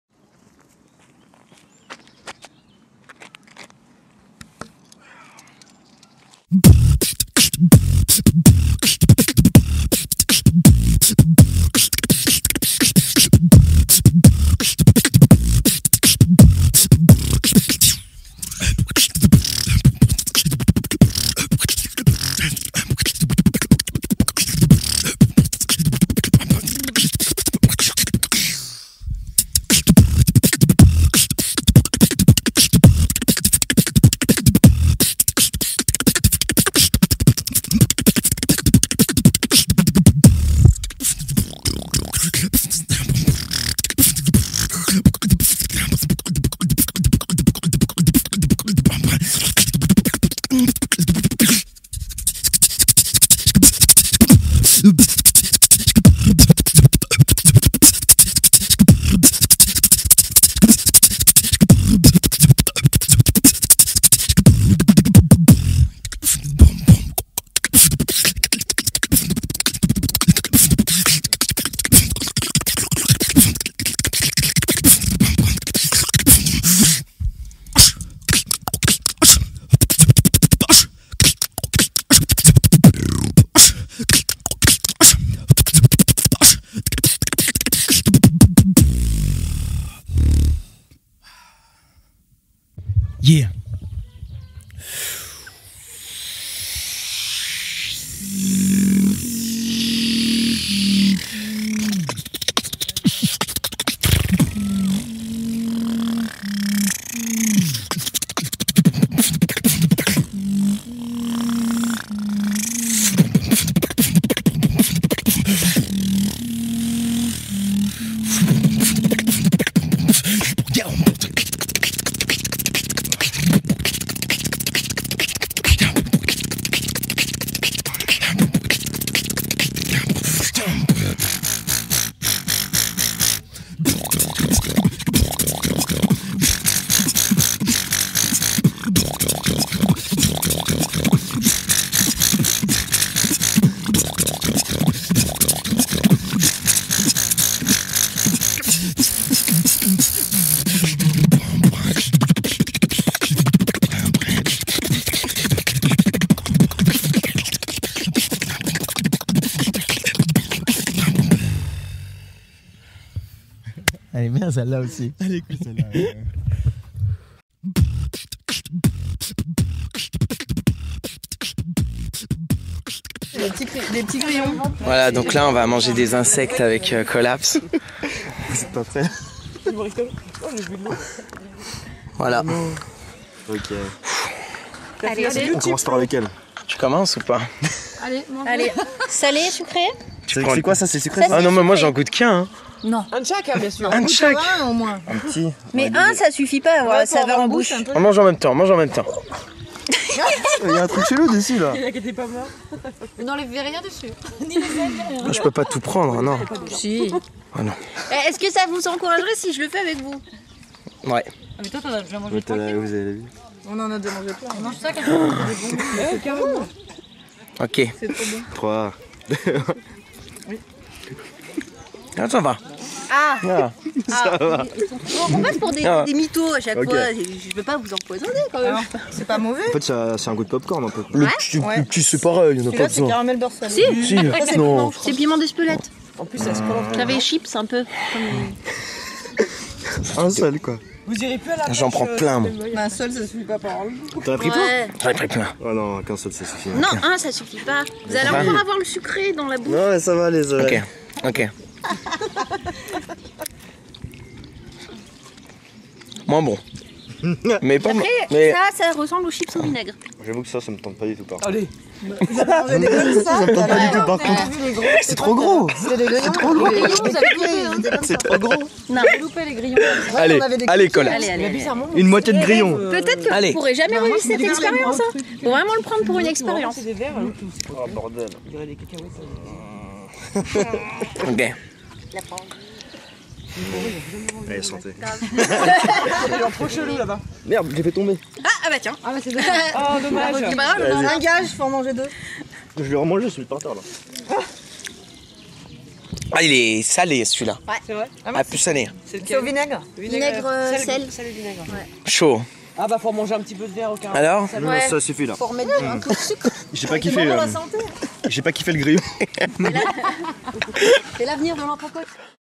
Aggressive Beatbox Freestyle | Alem & Colaps
Beatbox Freestyle with Colaps!!!
A big pleasure to work with him, he has so much motivation, passion and an amazing technique!! He is a real performer. I am very happy to share with you this long freestyle!
Colaps is the New French Beatbox Champion !!

Camera : Canon Legria mini ☓
Sound : Shure Sm7b In Zoom h4n Pro
Recording in my country : aiguebelette.

Alem.